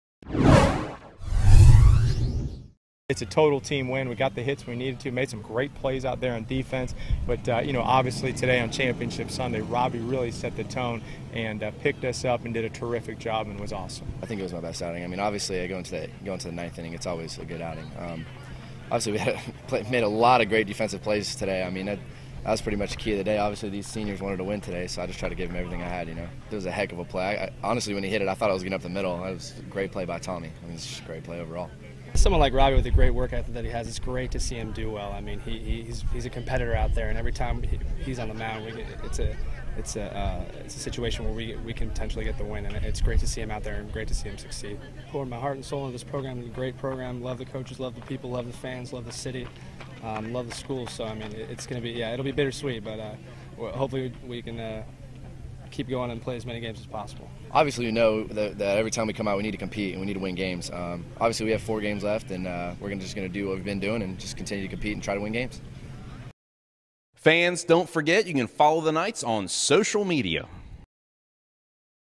RECAP: UCF Baseball vs. Memphis (5/10/15)
UCF head coach Terry Rooney, RHP Robby Howell and SS Dylan Moore recap the Knights' 4-1 win to clinch the series vs. Memphis.